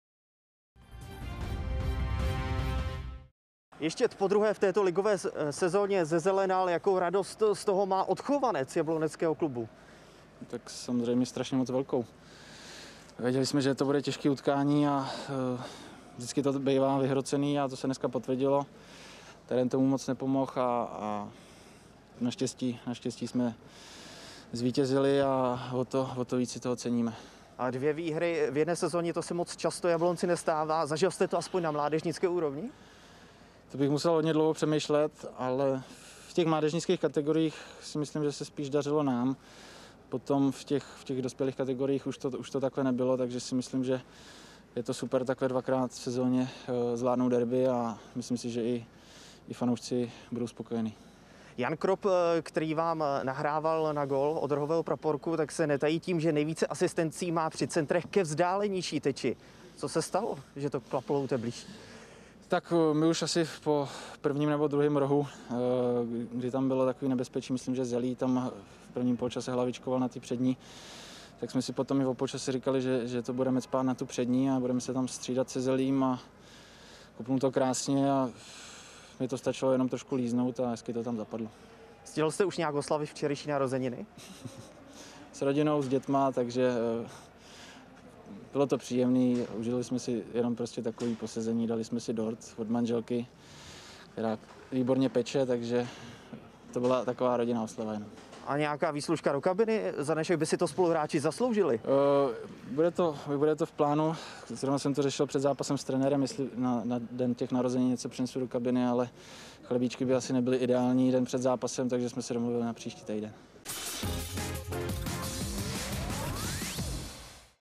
Jablonecký Kubista si dal k narozeninám vítězný gól v derby s Libercem
O výhře Jablonce v derby s Libercem rozhodl Vojtěch Kubista, který dával vítězný gól na 2:1. Dal tak sobě i týmu hezký dárek ke svým pátečním narozeninám. "Mám velkou radost, vždy to bývá vyhrocené. Naštěstí jsme to zvládli a o to víc si toho ceníme," usmíval se.